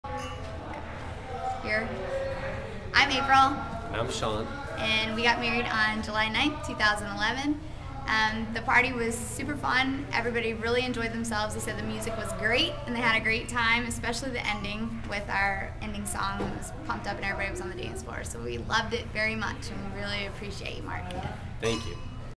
April Reynolds and Shaun Michael 07 09 2011 at Orange County Regional History Center
Yet another super wedding at the Orange County Regional History Center!